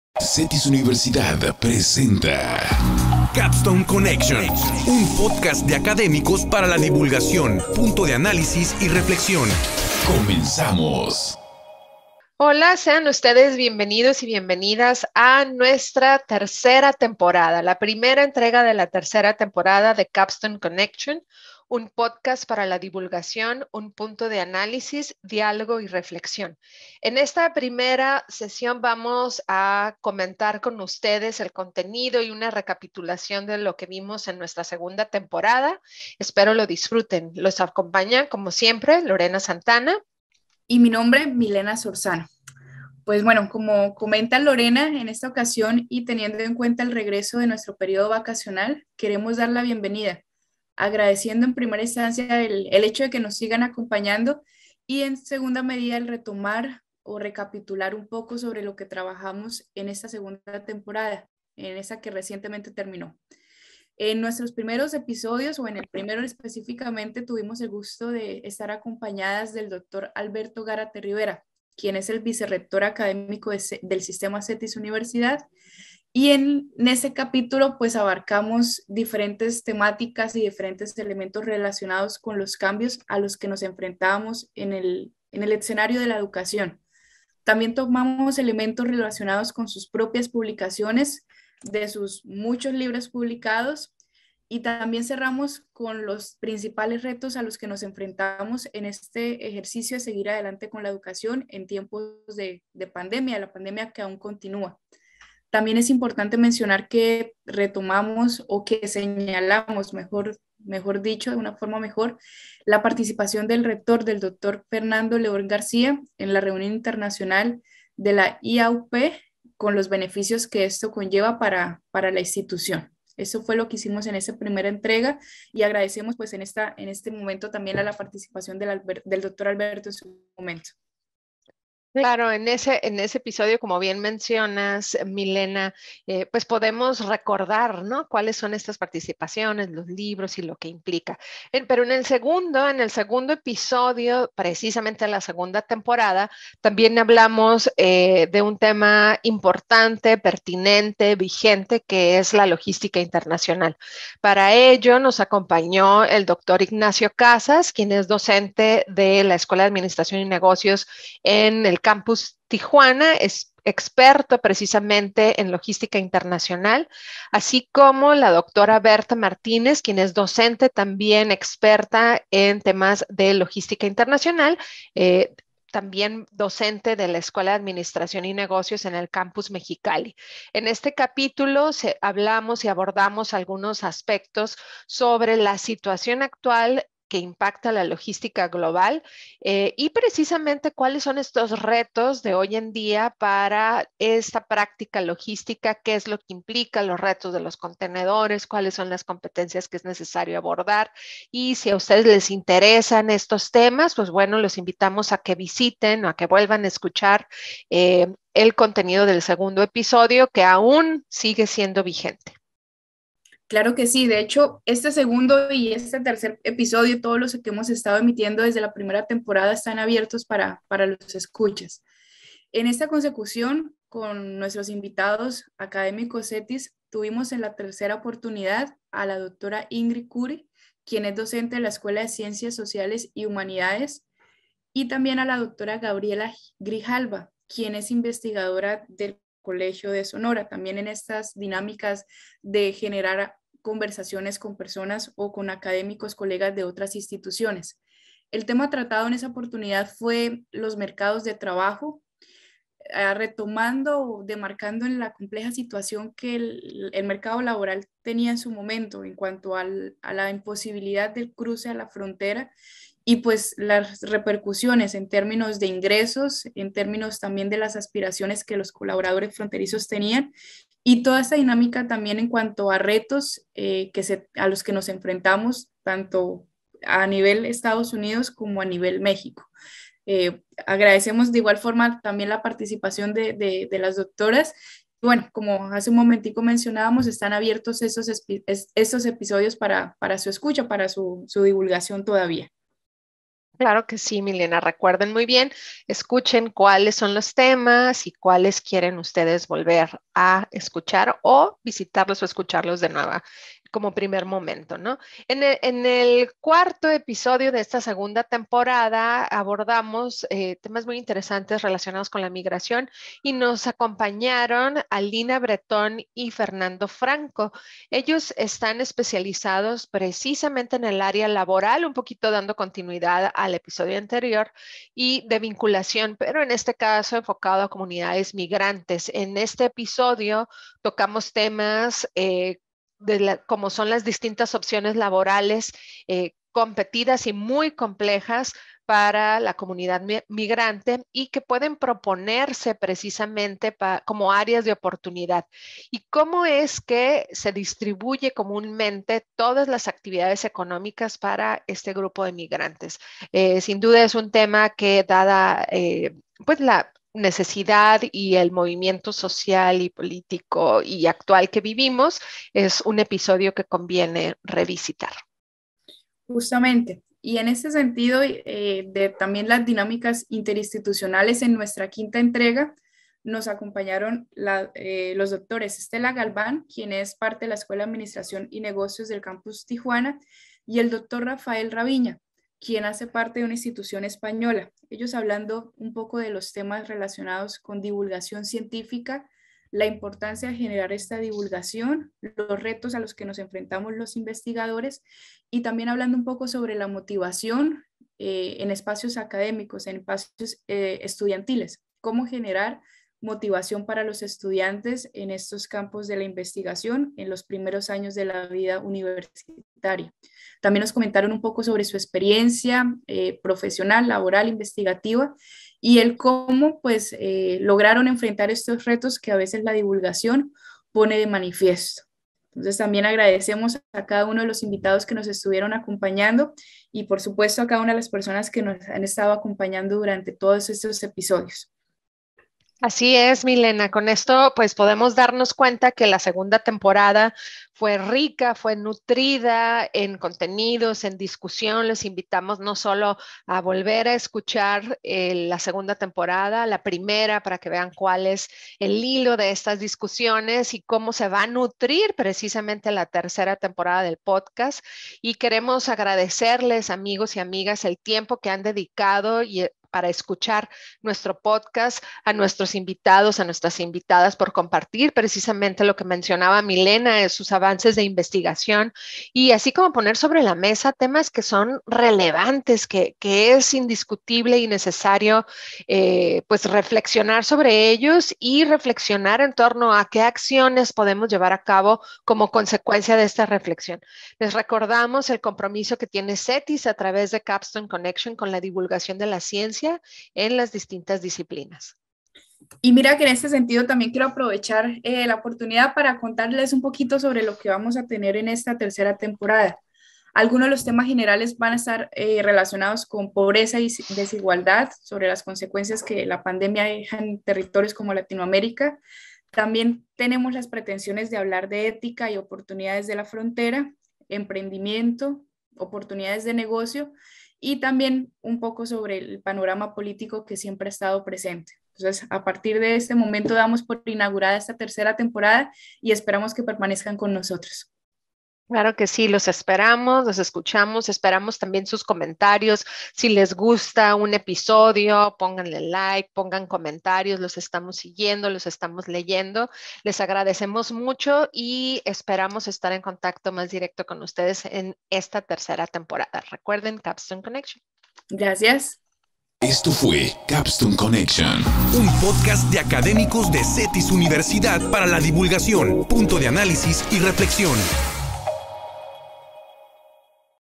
Capstone Connection: Listos para la T3 VIDEO T3E1 mp3
En este 2022 en Capstone Connection seguiremos compartiendo y comentando los trabajos de investigación de la comunidad académica en CETYS Universidad de las distintas escuelas, así como el trabajo de colaboración que hace con otras universidades e investigadores. Y por supuesto un análisis de temas actuales con un enfoque académico. Comenzamos esta tercera temporada.